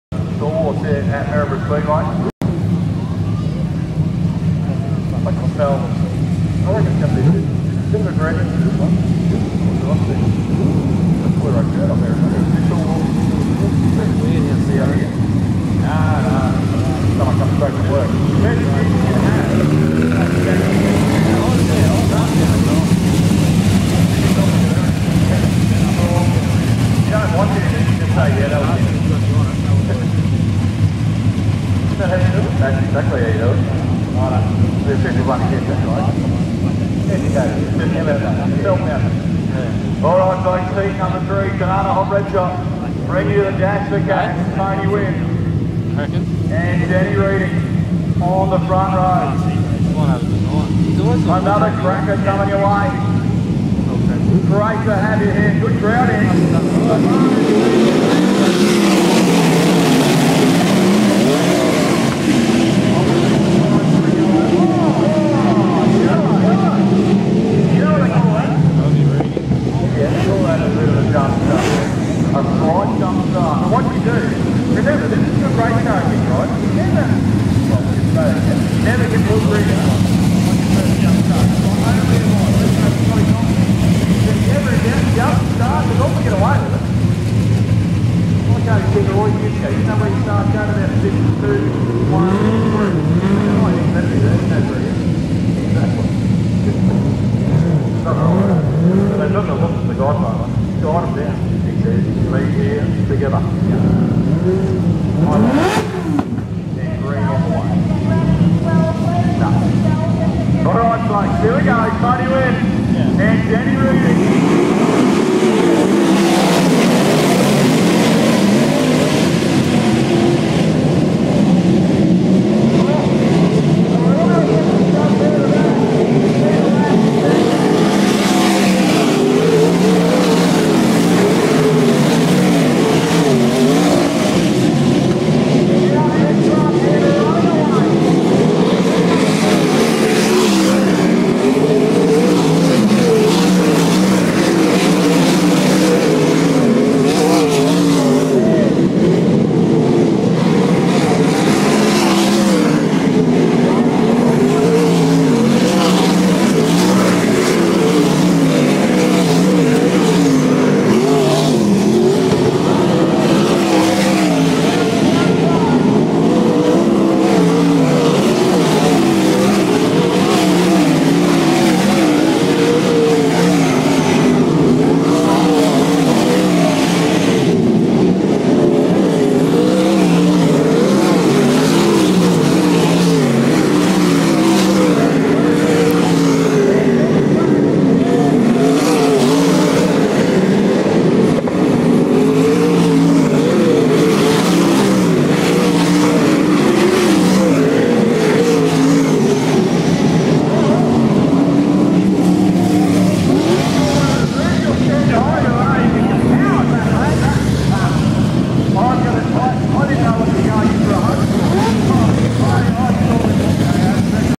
Maryborough Speedway 12.11.11 Heat 3 Sprintcars
Heat 3 of the Sprintcars at Maryborough Speedway when the massive accident occured in the A-Main.